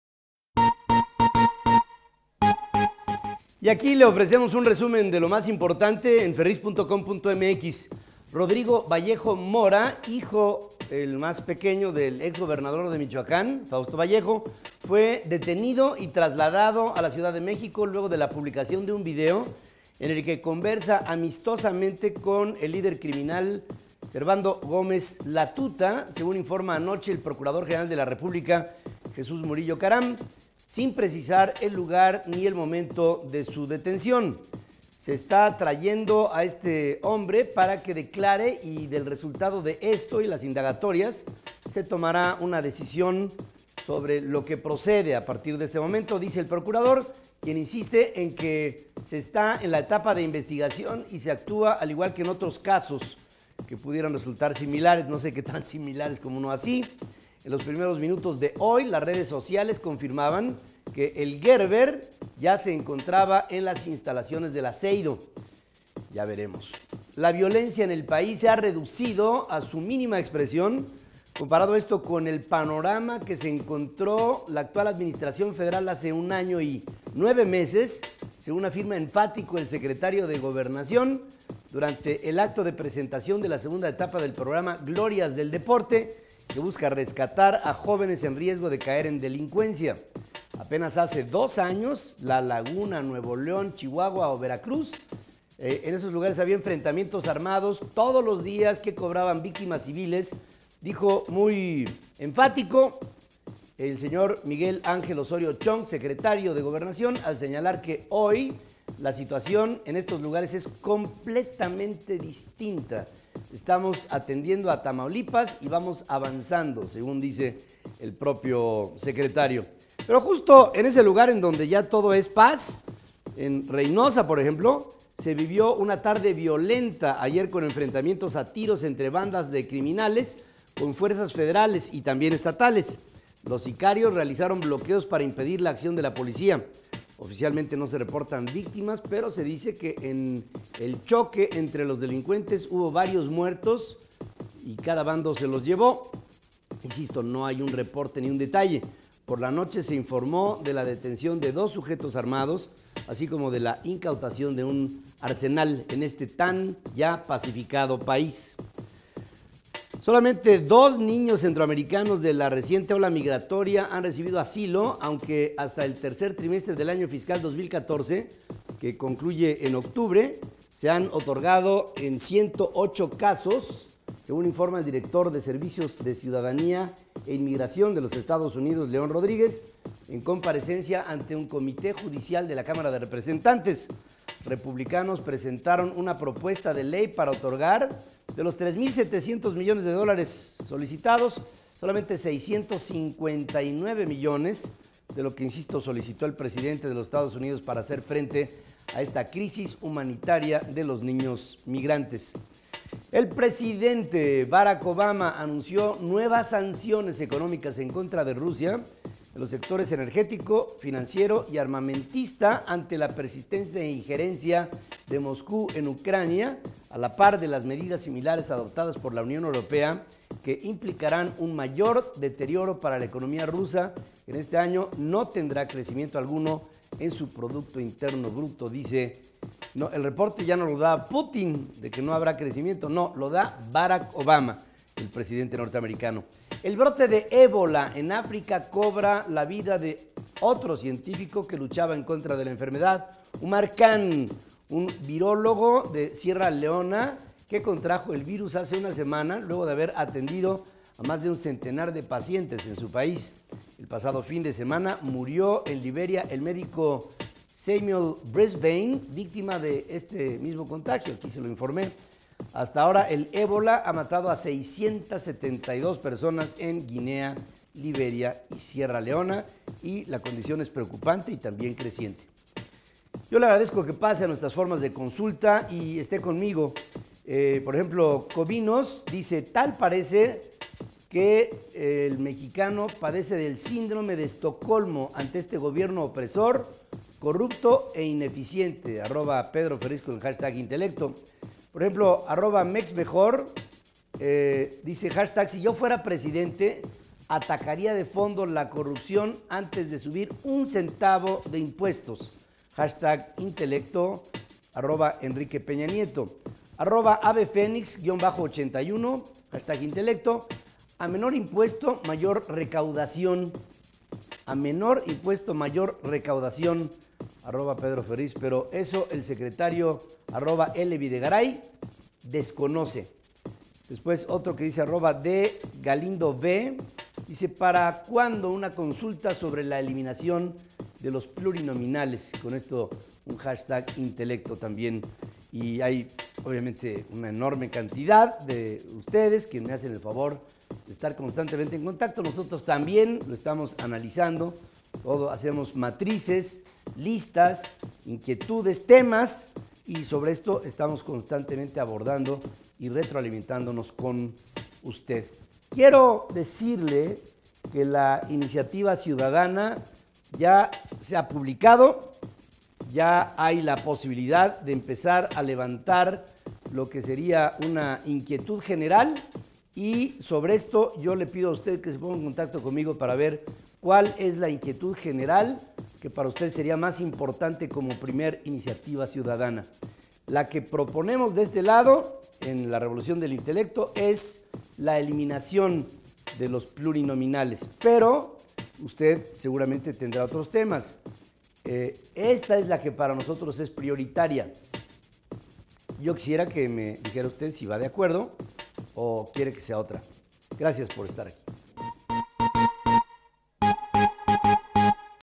Resumen de noticias
Rodrígo Vallejo Mora fue detenido y trasladado a la CDMX. La violencia en el país se ha reducido a su mínima expresión: SEGOB. En Reynosa se vivió una tarde violenta con enfrentamientos entre bandas criminales con fuerzas federales. Éstas y más noticias aquí.